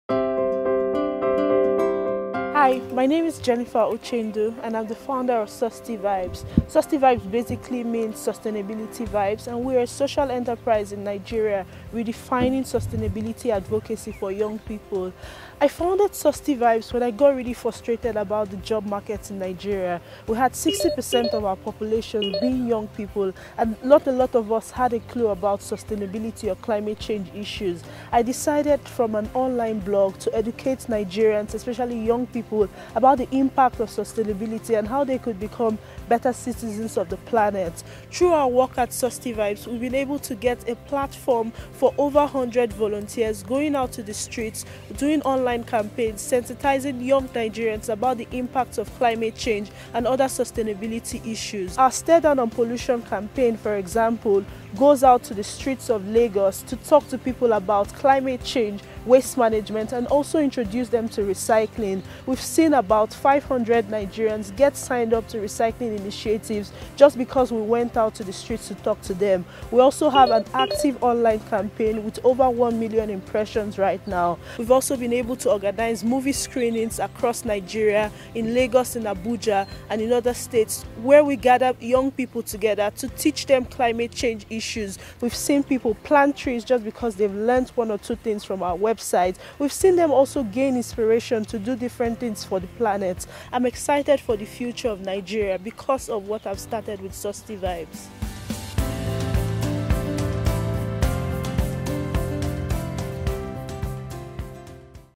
Jennifer, Nigeria | Global Youth Video 2017 | Climate Friendly and Resilient Cities
One of the 20 films shortlisted for the 2017 Global Youth Video Competition - Climate Friendly and Resilient Cities category.  Find out more about the competition on our website.

One  view = one vote